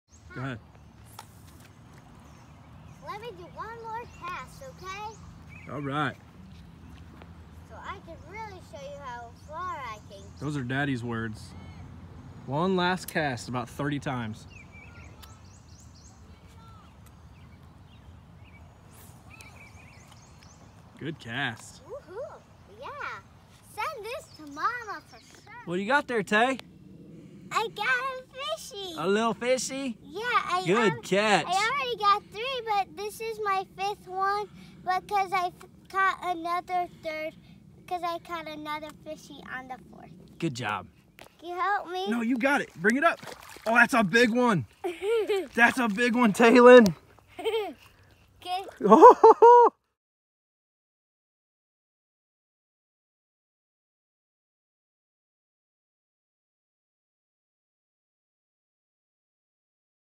Taelyn Goes Fishing
Tae catches some crappie monster crappie